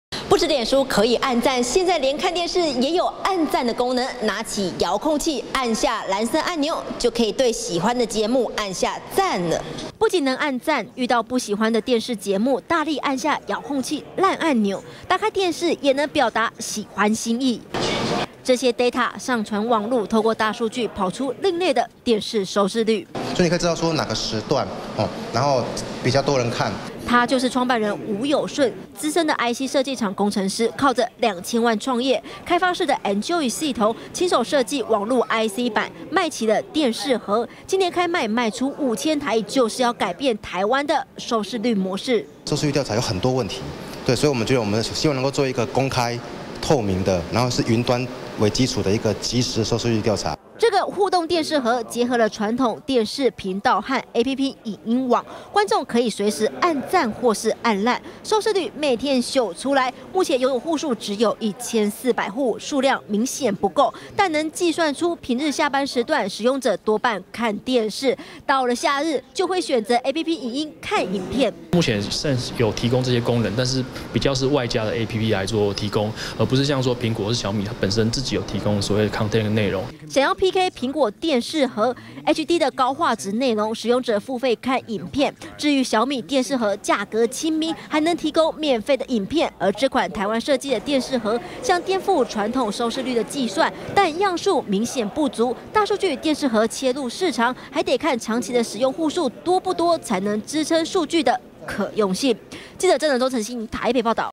20150504中天新聞 工程師設計電視盒 電視內容也能按讚
台灣半導體工程師，想要改善台灣電視收視率環境，長期受到特定調查機制綁架，設計一款新型態的電視盒，連結傳統電視和ＡＰＰ影音，觀眾可以在收看的電視節目中，按下讚或是爛，透過大數據跑出新型態收視率！不過推出來的價格要價３千多，高出小米電視盒和蘋果電視盒。